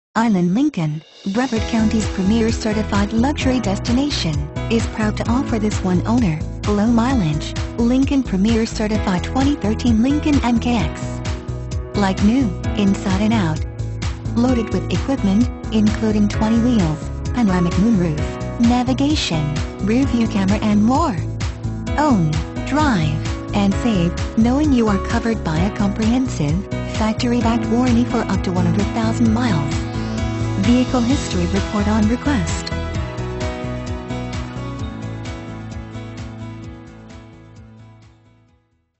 2013 LINCOLN MKX Merritt Island, FL P20389
2013 LINCOLN MKX Merritt Island, FL
Stock# P20389

Island Lincoln
1850 East Merritt Island Cswy

Island Lincoln, Brevard County's Premier Certified Luxury Destination, is Proud to Offer this 1-Owner, Low Mileage, Lincoln Premier Certified 2013 Lincoln MKX! Like New, Inside and Out! Loaded with Equipment, Including 20'' Wheels, Panoramic Moonroof, Navigation, Rear View Camera and More! Own, Drive, and Save, Knowing You are Covered by a Comprehensive, Factory Backed Warranty for Up to 100,000 Miles! Vehicle History Report on Request